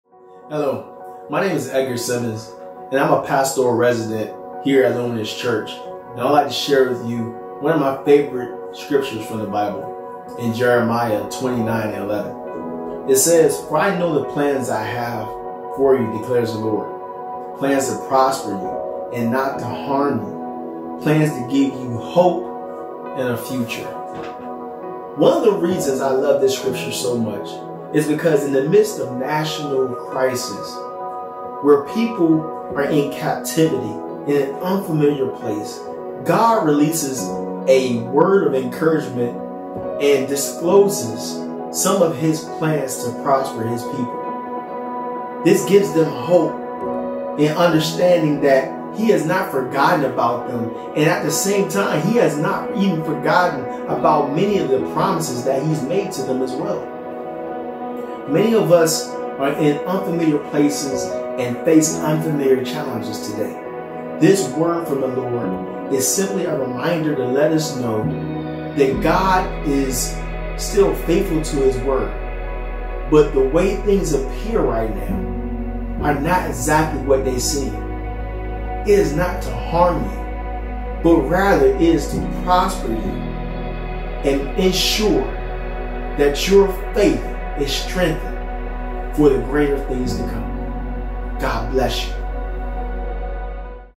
Speak Life // Daily Life Talk II
Pastor Edgar Simmons encouraging in the midst of trial.